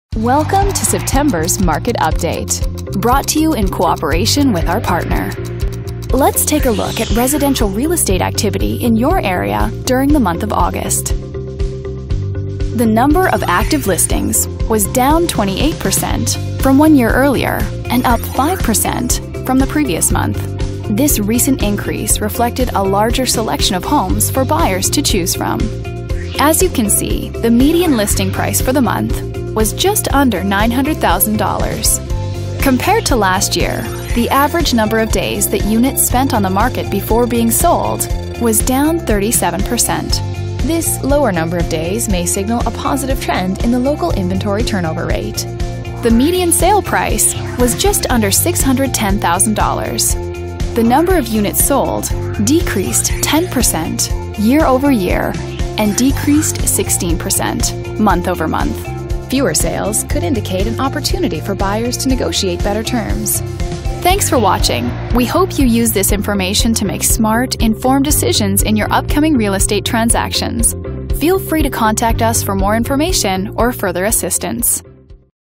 Evergreen/Kittredge/Indian Hills,CO, Real Estate Market Update from REMAX Alliance,September, 2018
REMAX Alliance Video Update recapping market activity within Evergreen/Kittredge/Indian Hills, CO, during the month of September, 2018